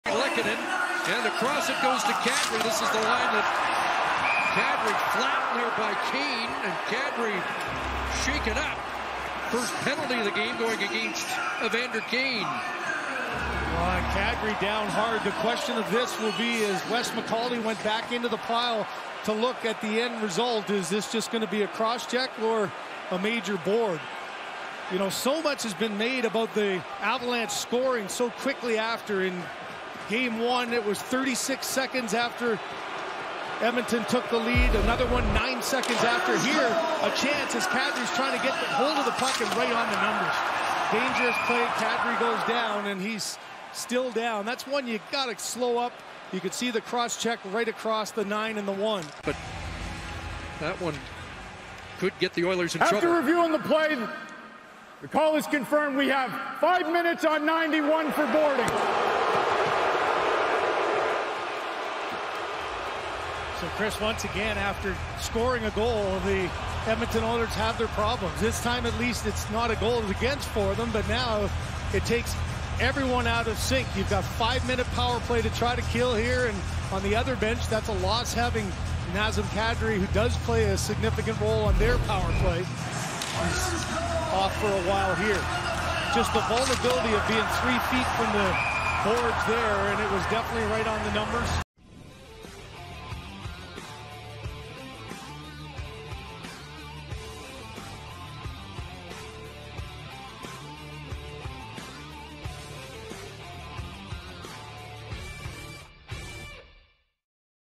Evander Kane Receives Major Boarding Penalty For Hit From Behind On Nazem, Kadri
Watch as Edmonton Oilers' Evander Kane recieves a 5-minute major boarding penalty for hitting Colorado Avalanche forward Nazem Kadri from behind into the end boards.